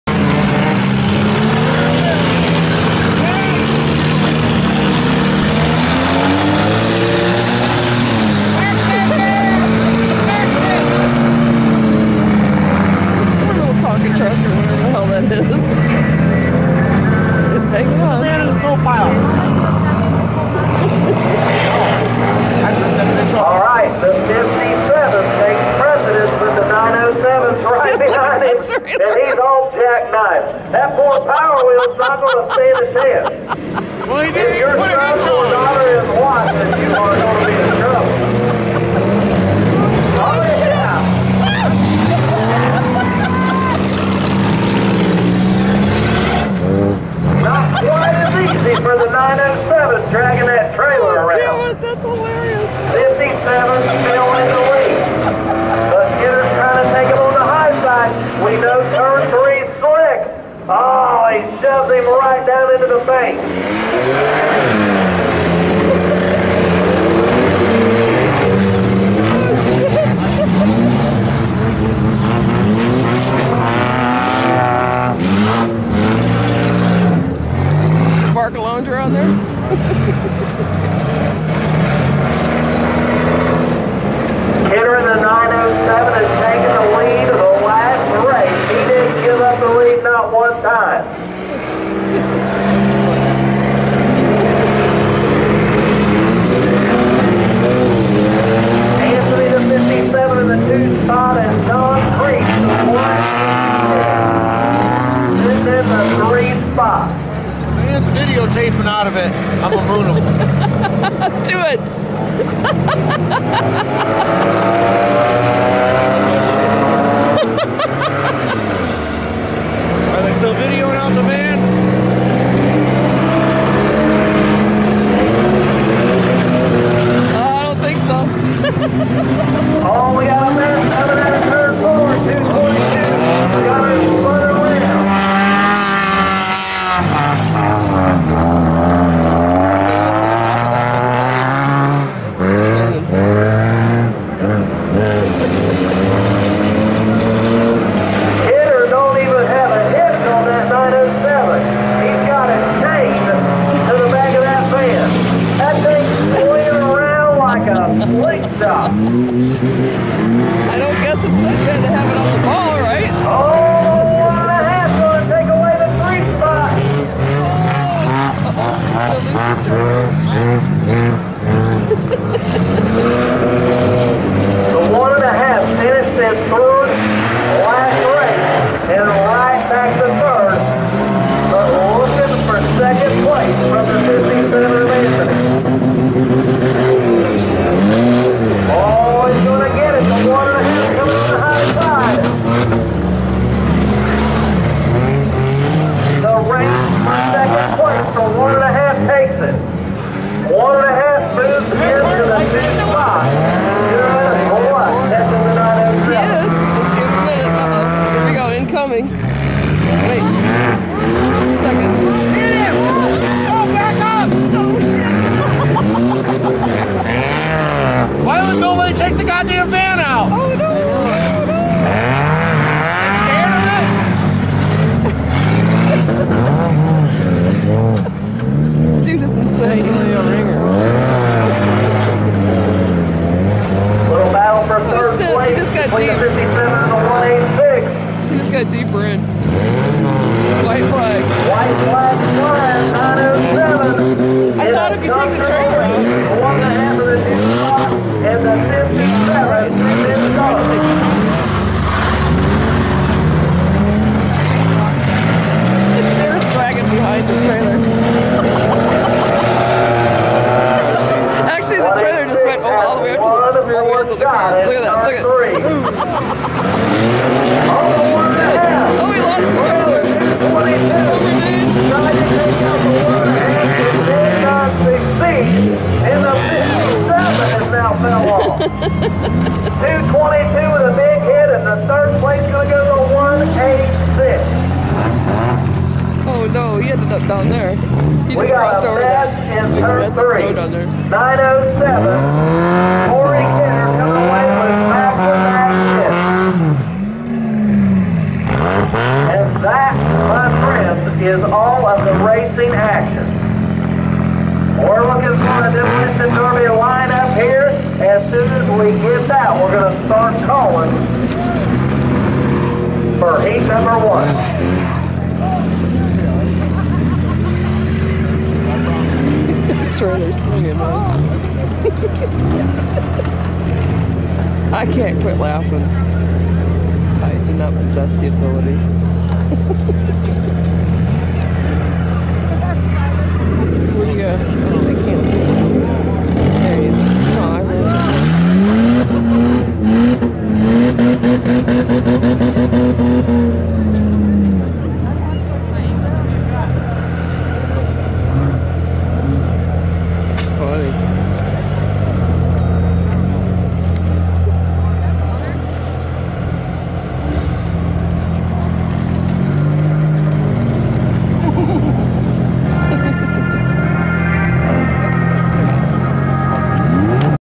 2/13/21 M40 Speedway, Jones, MI Valentines Crush Trailer Race! 😃
2/13/21 M40 Speedway, Jones, MI Valentines Crush
Trailer Race! 😃